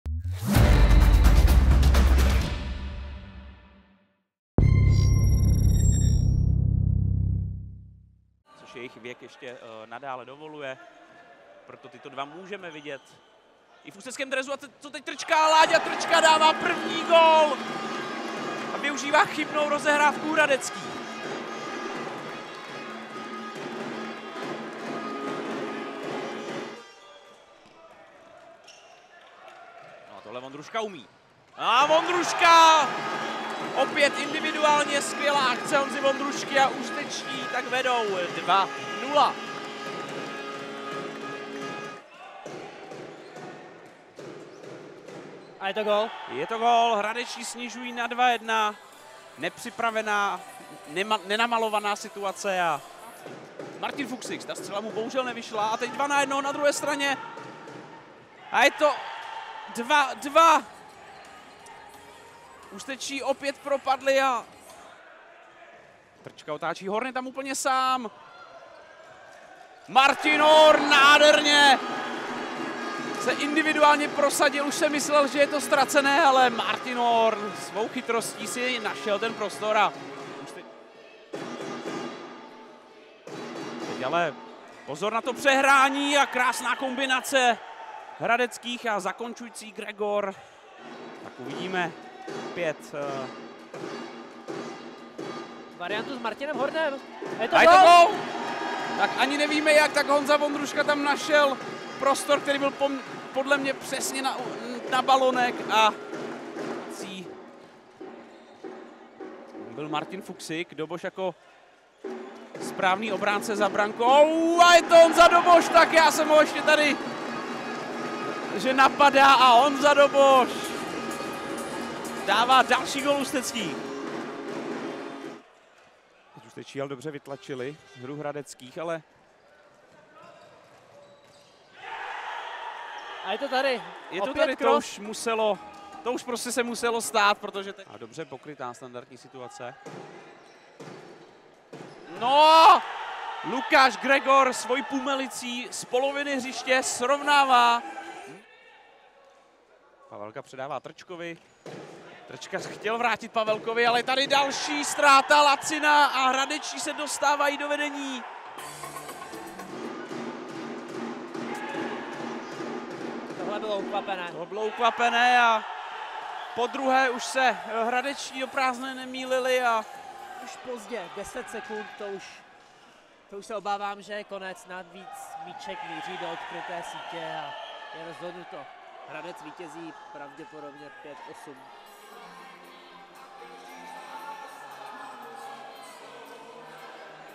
Sestřih utkání: Florbal Ústí - FBC Respect Hradec Králové 5:8
Sezóna 2024/2025 | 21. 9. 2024 | 3. kolo

Vyrobil Martin Harmach